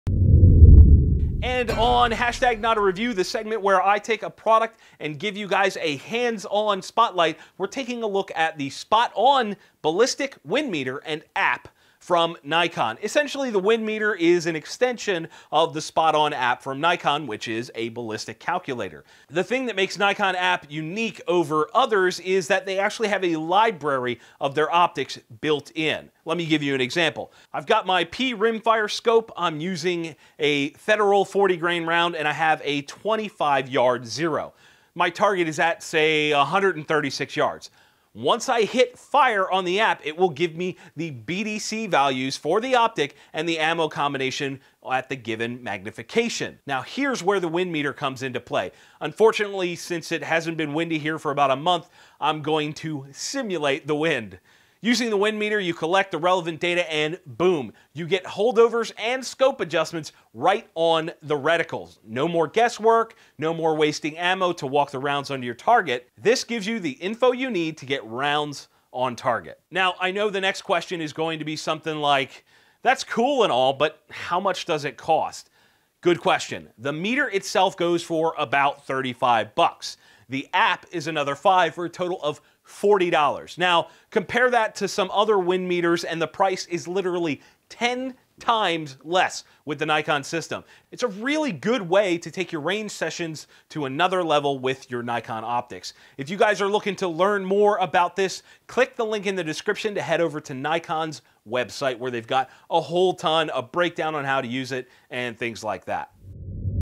Nikon Spot-on Wind Meter and App - NotAReview!
Today on NotAReview, Jon Patton is taking a closer look at the Nikon Spot-on Wind Meter and App.